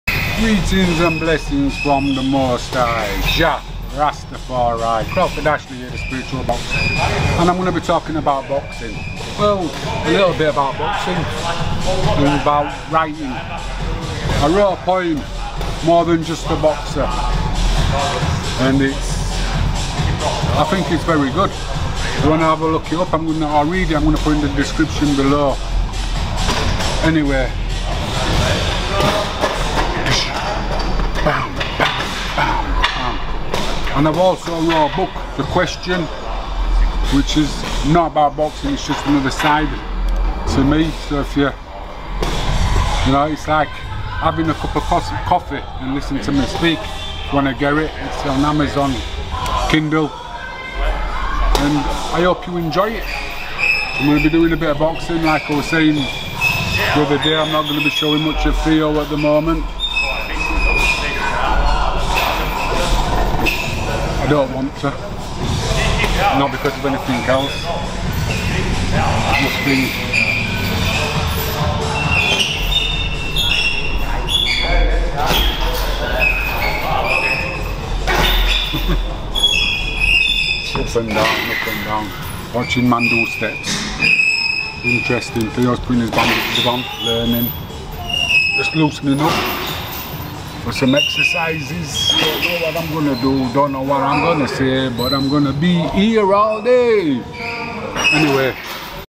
more than a boxer
link to my book The Question

Are You That Boxer?

I am more than a boxer
That boxer you only see.
The boxer the world sees as me.

I am more than just...
I am a writer, dancer and a poet too,
I’m so deep and quite unique.

I am more than just a boxer listen to me speak,
For there is sense in the nonsense if only you’d seek.

I am more than a boxer can’t you see.
I am a tree with roots in the earth
so deep you can’t see.
I am the leaf on the tree
that the tree sets free.

I am the caterpillar you don't see
that turns into a butterfly
which catches your eye
I am the bee that pollinates
 the tree that sets me free.
Can’t you see I am more than a boxer

I’m more than a boxer only a few will see.
Im deeper than the ocean
 and rougher than the sea.
I am the thunder storm
the lighting that flashes.
I shout out to the masses
I’m even good at poetry.

Thank you, C